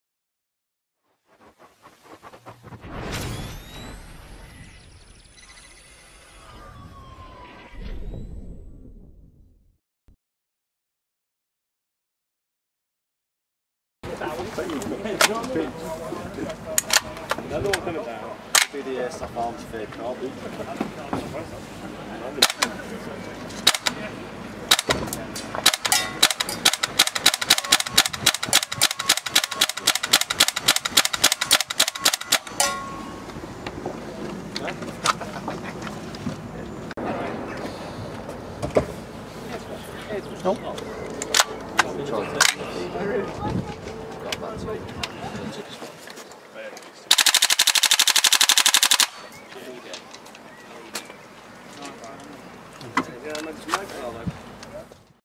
GHK G5 Gas Blowback Test Fire
Test firing of the GHK G5 Gas Blowback Rifle during the Airsoft Arms Fair 7 held last 18 May 2013. Test firing was done with the Airsoft Odyssey Crew.